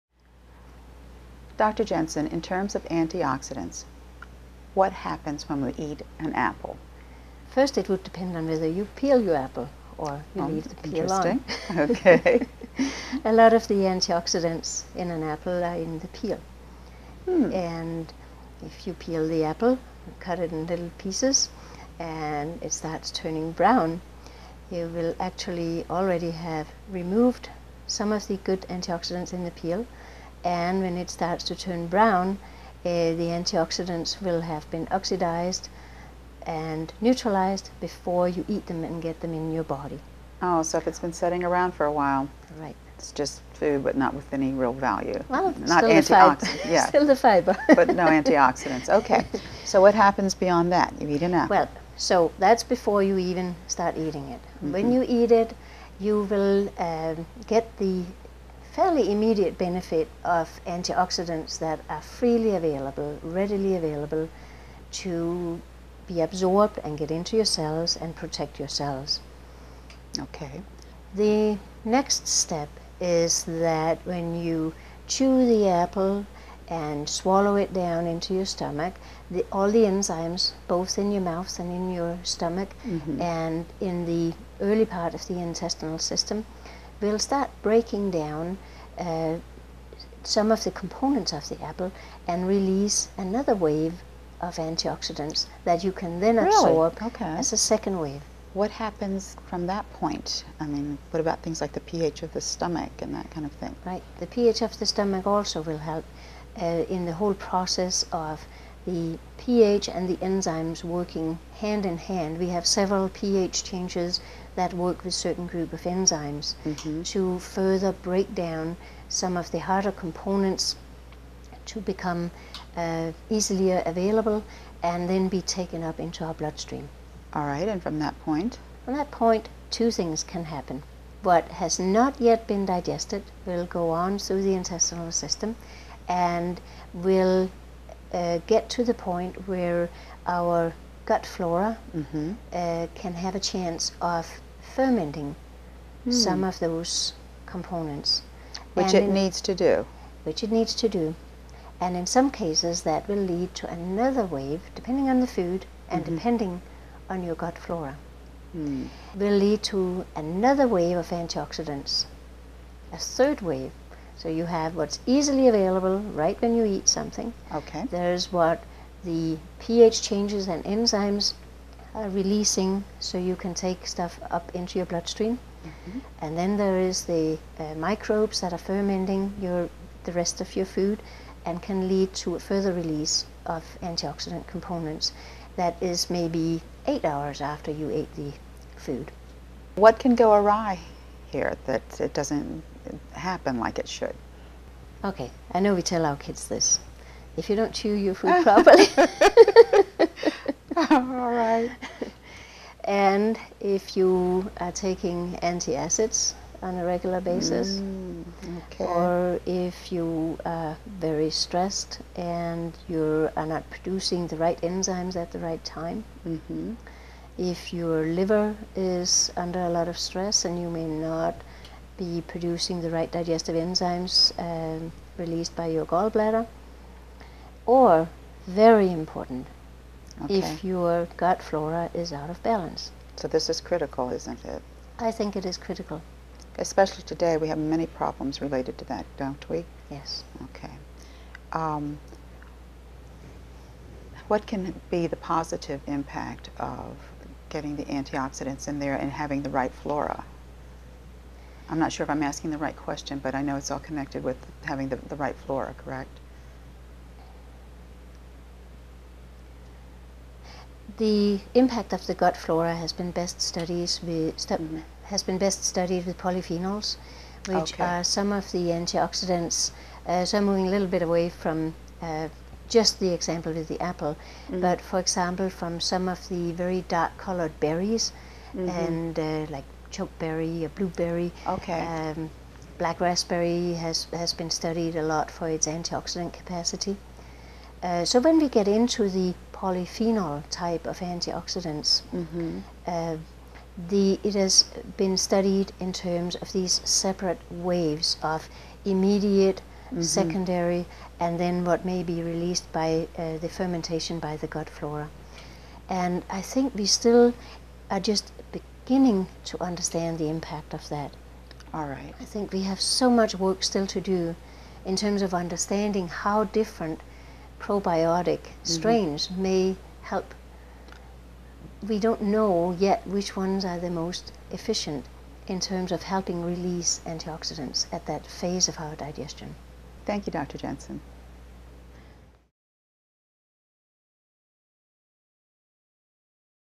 Maximizing Antioxidants
Dr. Gitte Jensen talks about how to get the most from your antioxidants.